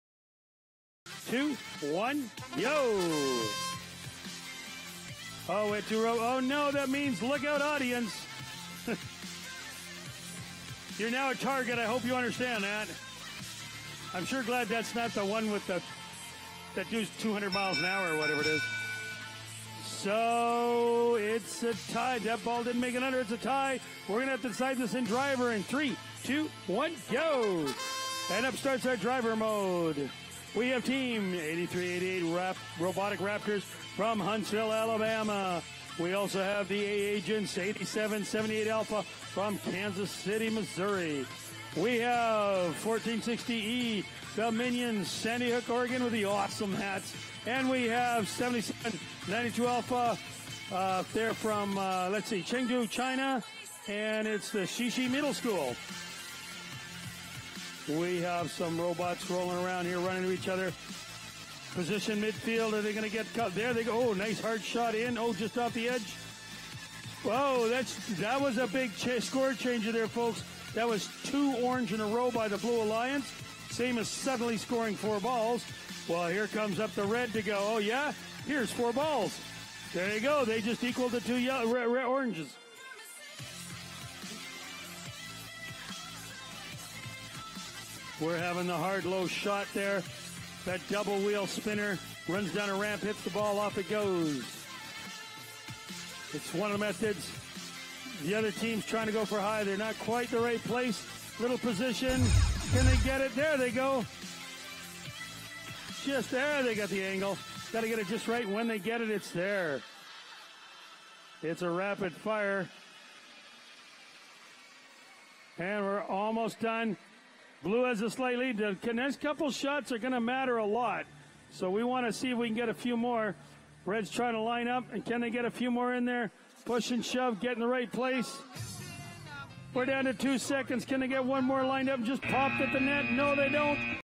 VEX Worlds 2016 - VRC Middle School - Opportunity - Qual 151 (8388 8778A) 104 vs 162 (1460E 7792A)
VEX Worlds 2016 - VRC Middle School - Opportunity - Qual 151

Red - Score 104:
8388 - Robotic Raptors (Meridianville Middle School, Huntsville, Alabama, United States)
||  Opportunity Rank:82 . Wins:2 . Loses:8  . WP:4 . SP:1015

8778A - A Agents (Barry School, Kansas City, Missouri, United States)
||  Opportunity Rank:28 . Wins:6 . Loses:4  . WP:12 . SP:1190

Blue - Score 162:
1460E - Minions (Sandy Robotics Club, Sandy, Oregon, United States)
||  Opportunity Rank:58 . Wins:4 . Loses:6  . WP:8 . SP:977

7792A - CD Shishi MSch A (Chengdu Shishi Middle School, chengdu, China)
||  Opportunity Rank:29 . Wins:6 . Loses:4  . WP:12 . SP:1183